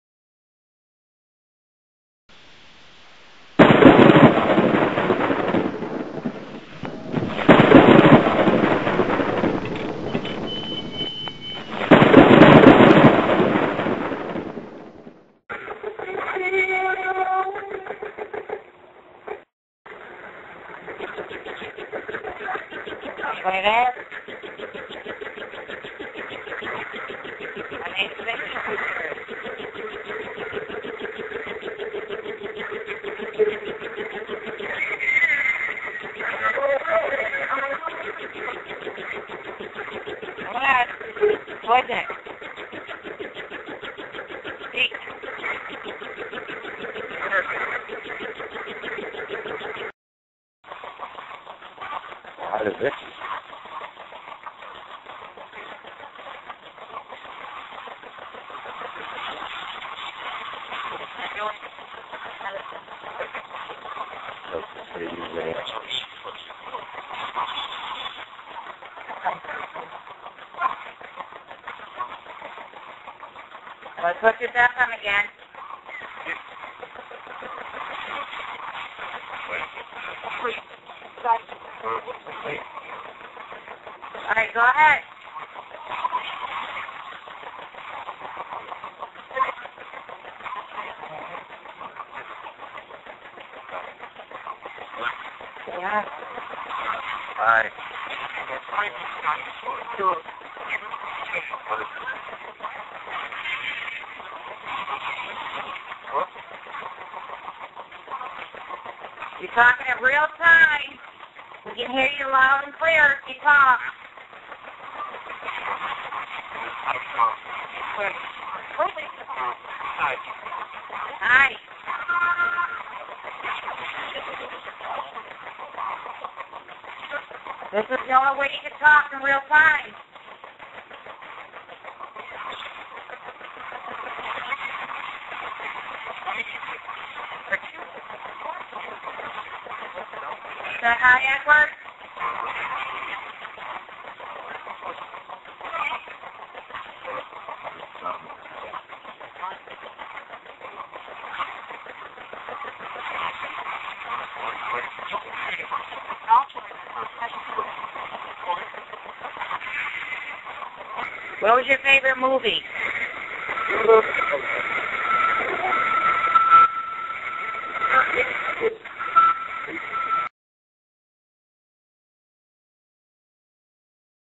Vicki's Ghost Box Session 1
This is a 12-469 Radio from Radio shack modified. I modified it myself with the help of UFO Geek. I Just followed his Instructions and It worked. Hope you like it.